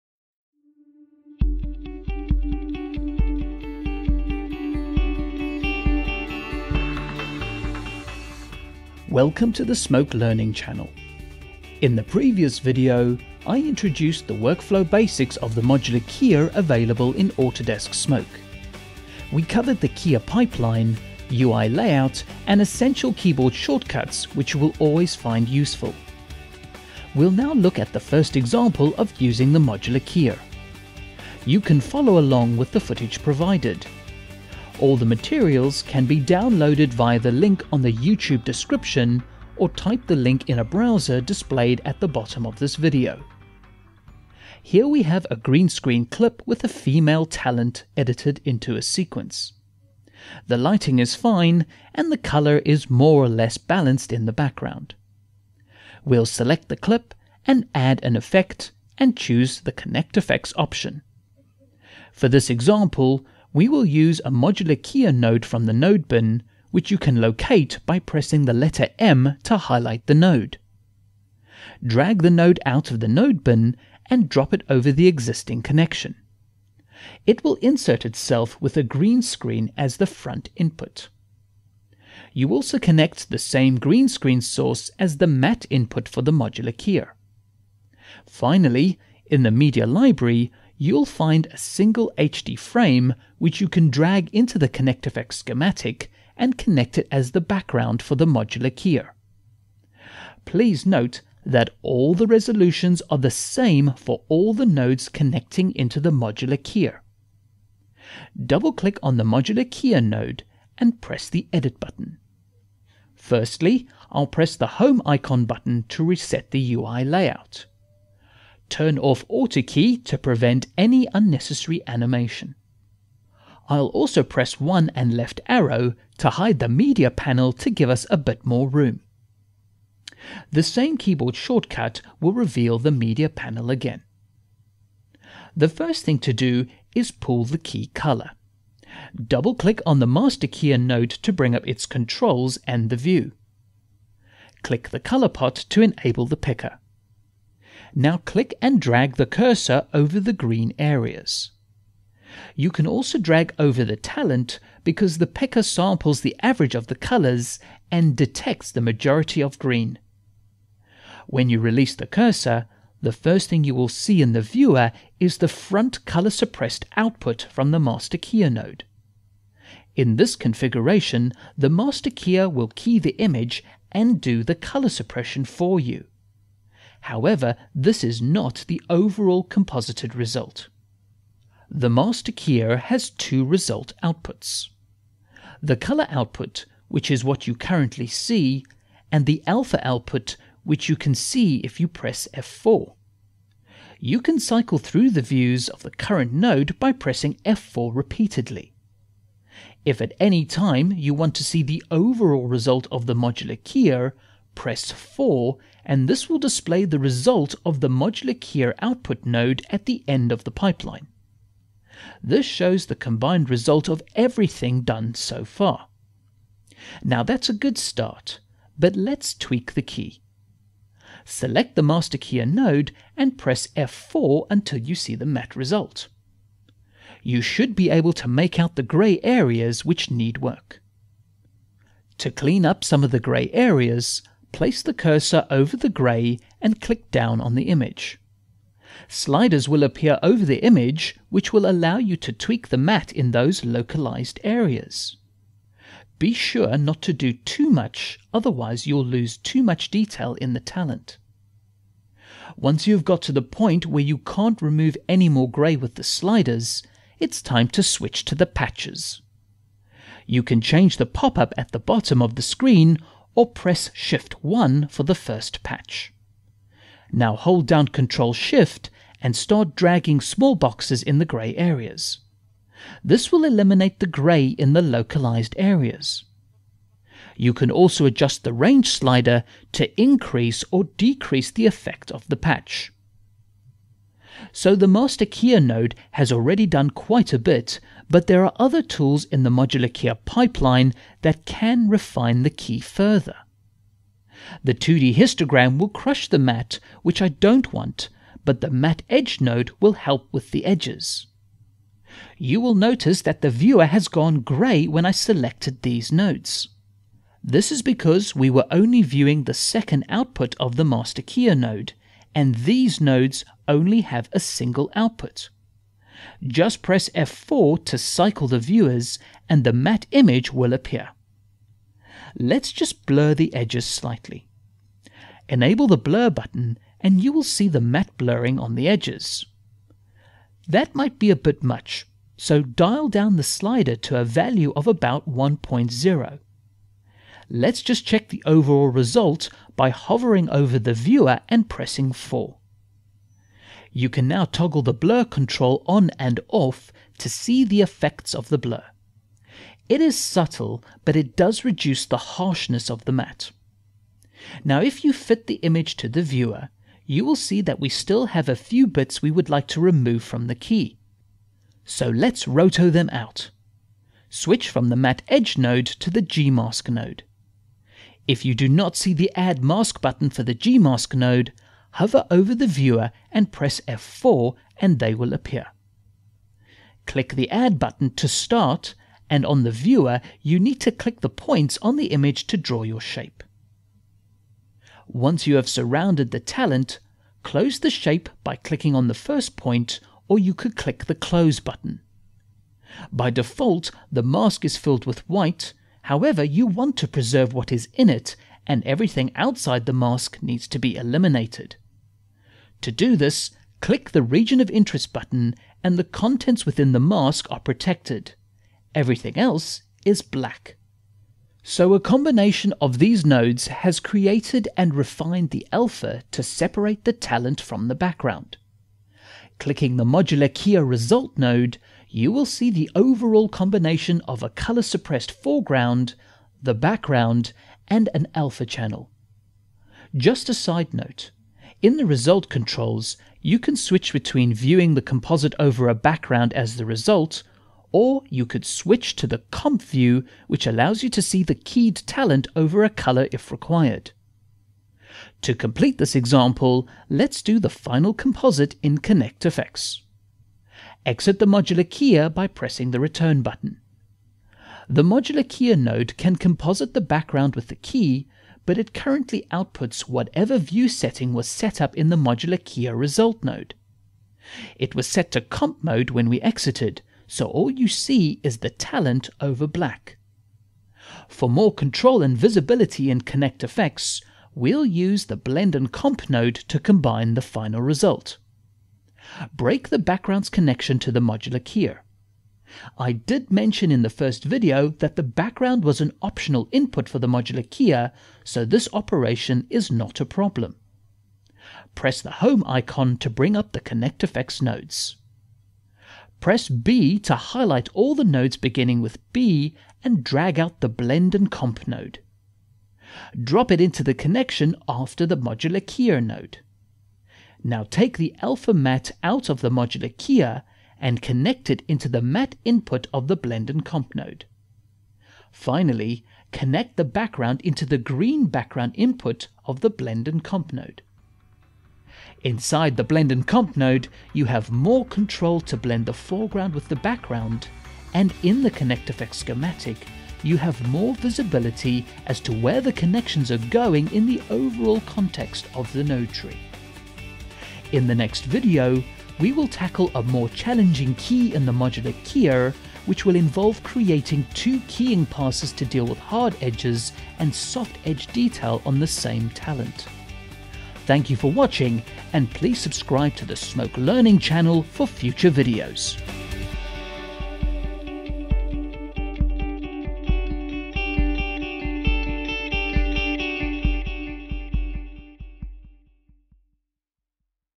The Modular Keyer - Part 2 - Basic Example
Level: Beginner
Recorded in: Autodesk Smoke 2013

This video series goes through the modular keyer in great detail.  Starting from the basic explanation all the way up to challenging examples and workflow "gotchas".

Part 2 goes through a basic example from beginning to end using the modular keyer.  Just using the standard setup, you'll learn how to key and refine a chroma key task.

For Information, Feedback and Comments:

Autodesk Area